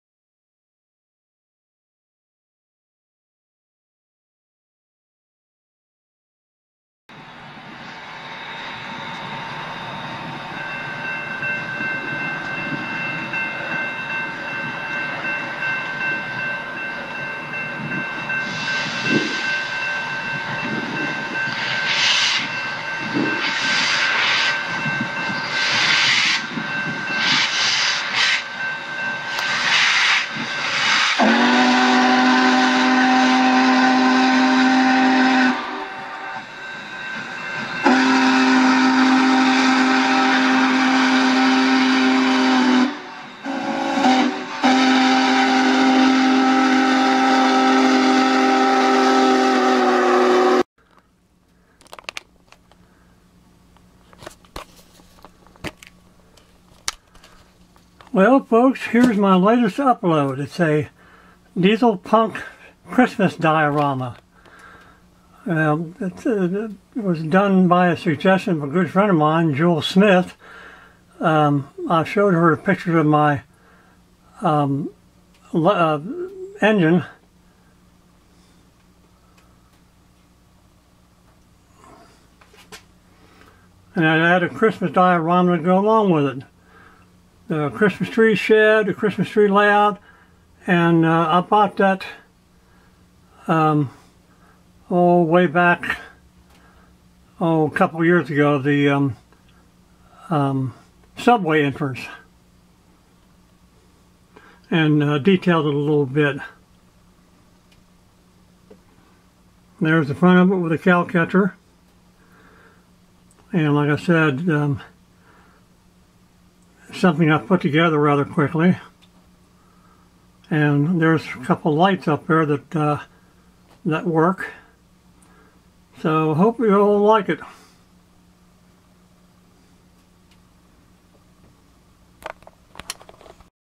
Steampunk Christmas Diorama
This was a diesel locomotive that I turned around backwards to make it look like a steam engine =and being into Steampunk ,couldn't help myself lol   In the Movie Maker I misspelled
Diorama and You Tube caught it